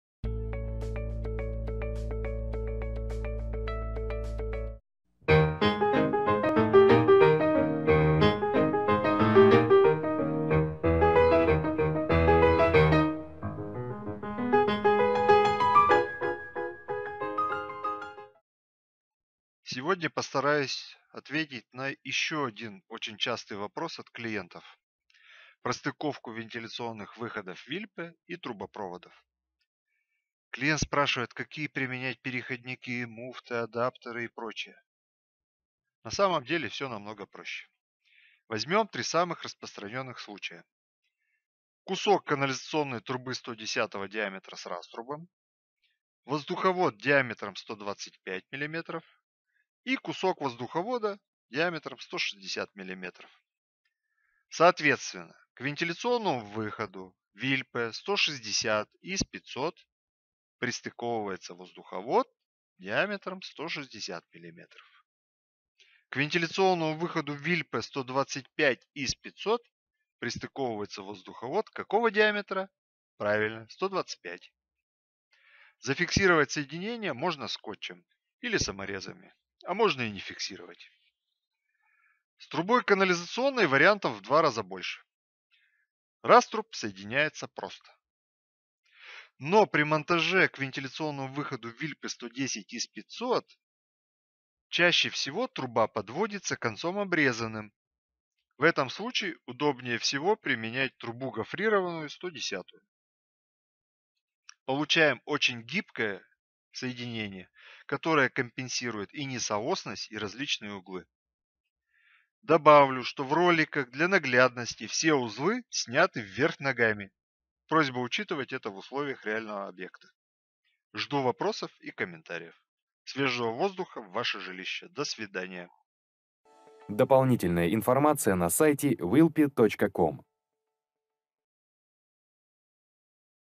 Как соединить вентиляционный выход VILPE с вентиляционным каналом .
Воздуховод с вентиляционным выходом стыкуется довольно просто:
К вентиляционному выходу Vilpe 160 - венттрубы 160 мм
К вентиляционному выходу Vilpe 125 - венттрубы 125 мм
К вентиляционному выходу Vilpe 110 - труба гофрированная Vilpe 110
Вентвыходы Vilpe можно приобрести в наших магазинах и на нашем сайте